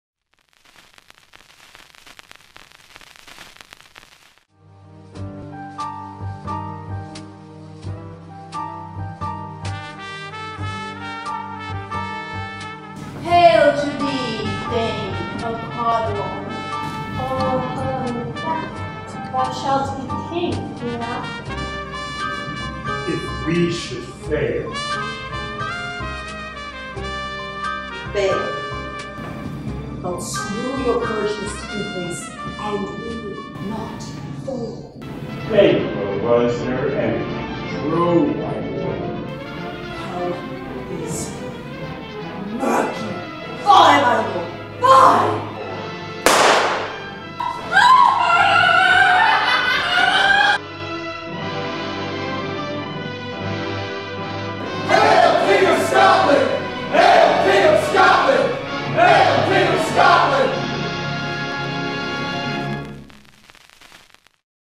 HITW Theater Presents: Macbeth
Hole in the Wall Theater is proud to present the classic Scottish tragedy of Macbeth by William Shakespeare, adapted by Mallory Spencer and Bill Arnold. We set the stage in the gritty, black-and-white, world of film noir as we follow Macbeth, prophesied by three witches to be king of Scotland. His path to the throne is a dark one, however, as what lays ahead of him includes betrayal, murder, and madness.

The show will run for four (4) weekends between March 15th and April 6th, with nine (9) performances on Fridays and Saturdays at 8 pm, with one (1) Sunday matinee, March 24th at 2 pm. The Friday, March 22nd is our annual Pay-What-You-Can Night.

Tickets will be available for purchase at the door ($20 general admission /$15 for students/seniors (62+) with ID), or you can secure your tickets in advance by purchasing them ($17 general admission/$12 seniors/students), or calling in a phone reservation to (payable at the door).